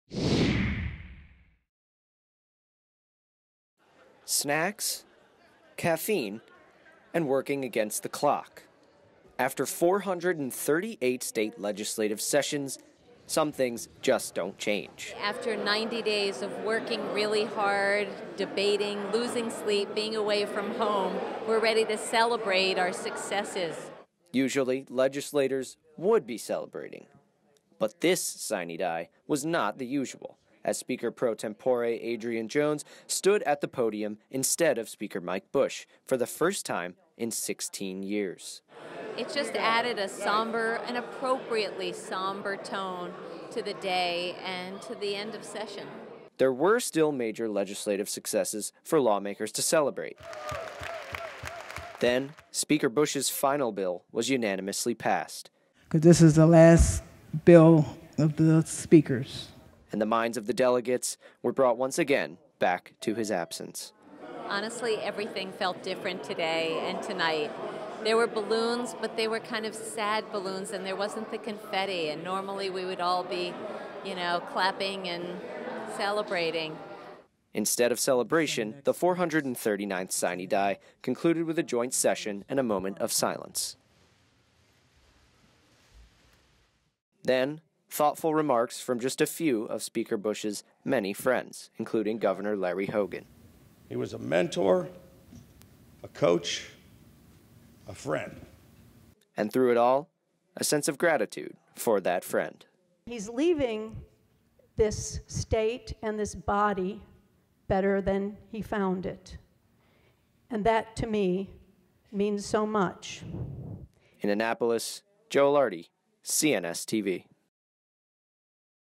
SINE DIE -- Lawmakers honor Speaker Michael Busch on the last night of the session
ANNAPOLIS, Maryland -- Maryland lawmakers worked through bills and meetings to close the 2019 state legislative session on Sine Die, despite the somber mood set by Speaker Michael Busch's death.